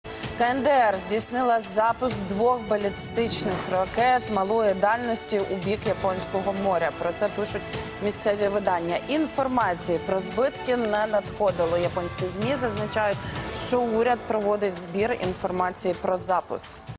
КНДР здійснила запуск двох балістичних ракет у бік Японського моря
Північна Корея 14 березня запустила дві балістичні ракети малої дальності у бік Японського моря.